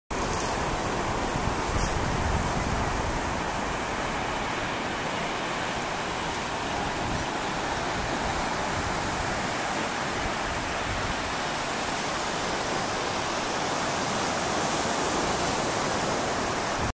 Scotts Head Dog Friendly Beach - Forster Beach - dogexplorer.com.au
Forster Beach is a dog friendly beach that can be accessed along Banksia Crescent.